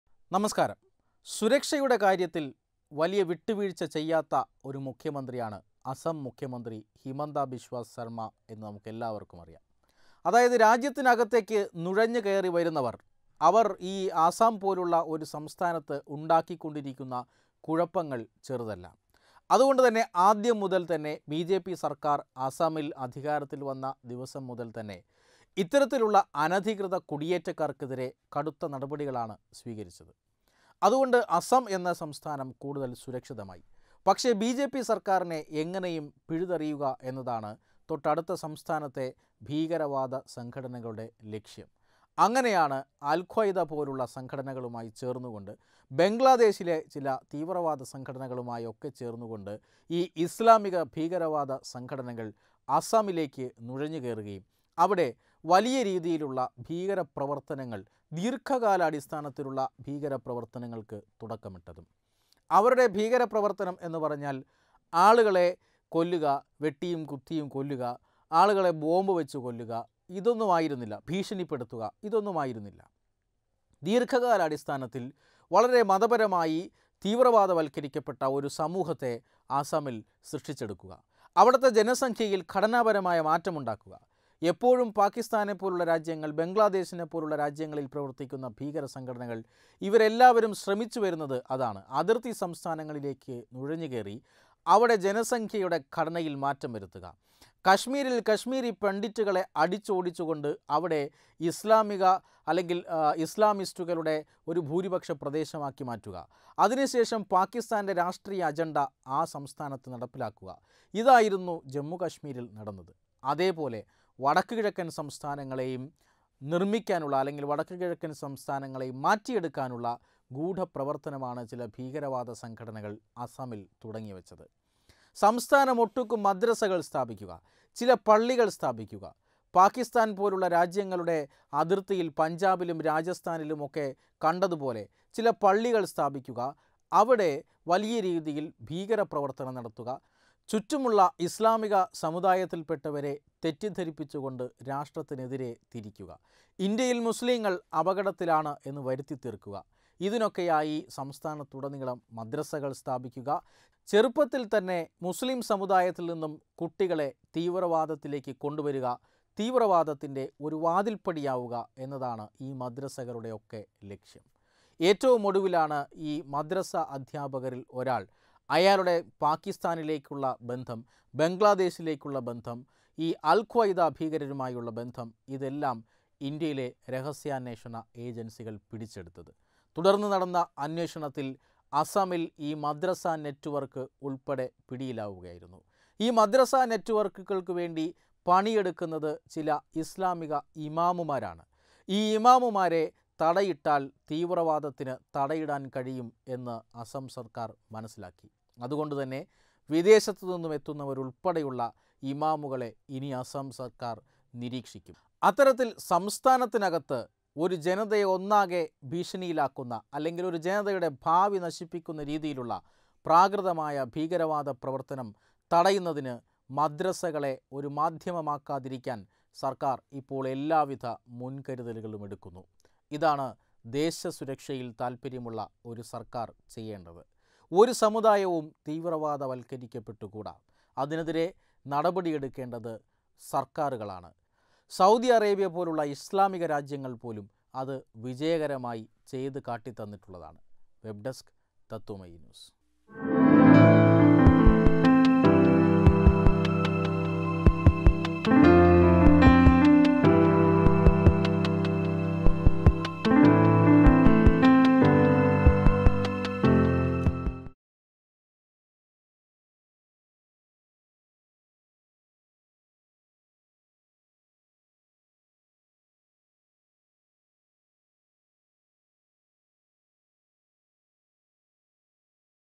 ഐ എസ്, ബംഗ്ലാദേശീ ഭീകരരെ തുരത്താൻ ഹിമന്ത ബിശ്വ | Assam
വിദേശ മത പ്രചാരകരെ നിരീക്ഷിക്കും മദ്രസാ ജിഹാദ് അടിച്ചൊതുക്കാൻ അസ്സം സർക്കാർ | Assam